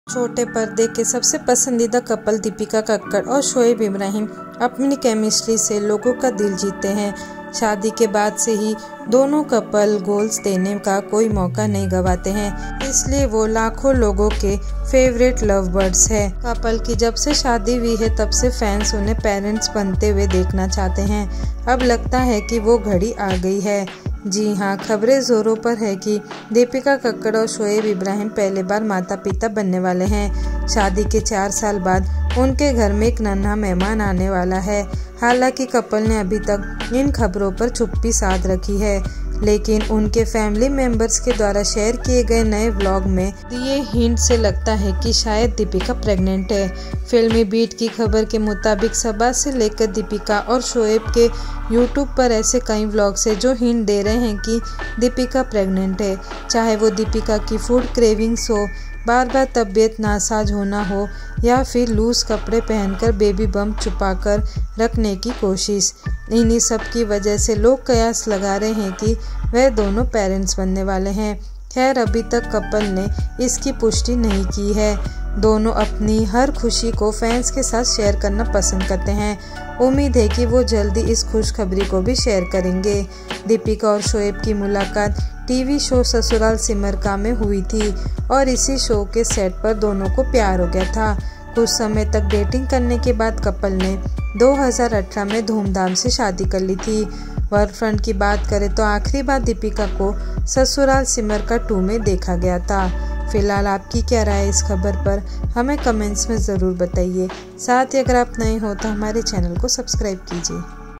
पहली बार मां बनने वाली हैं दीपिका कक्कड़? पति शोएब और ननद सबा ने दिया ये हिंट!
Dipika Kakar Pregnancy Rumors: टीवी कपल दीपिका कक्कड़ और शोएब इब्राहिम के जल्द ही माता-पिता बनने की खबरें सामने आ रही हैं. अनुमान लगाया जा रहा है कि, दीपिका प्रेग्नेंट हैं. जानें क्यों?